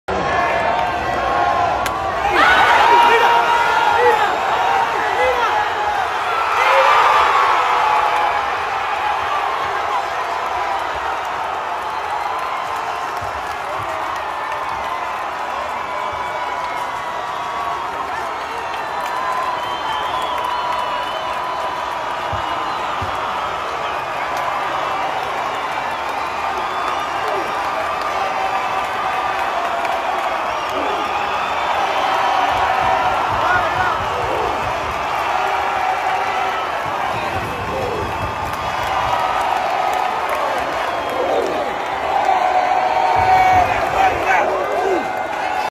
Yasiel Puig le da la Victoria a Tiburones de la Guaira con inmenso Cuadrangular.
yasiel puig